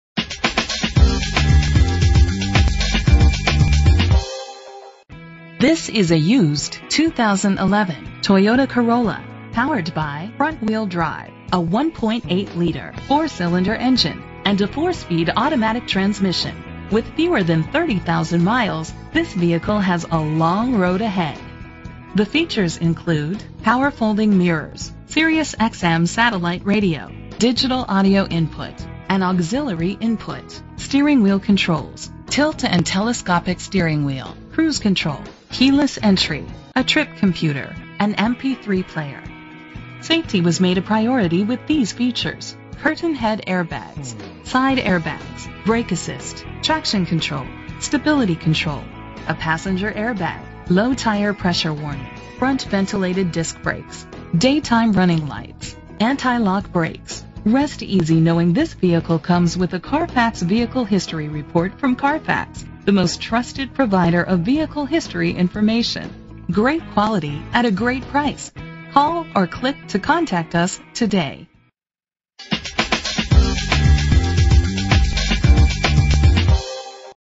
2011 Toyota Corolla - Elmhurst IL
Year: 2011
Make: Toyota
Model: Corolla
Trim: LE
Engine: 1.8 liter inline 4 cylinder 16 valve
Transmission: 4-Speed Automatic
Color: Super White
Mileage: 28685

Address:
440 W Lake St, Elmhurst, IL 6012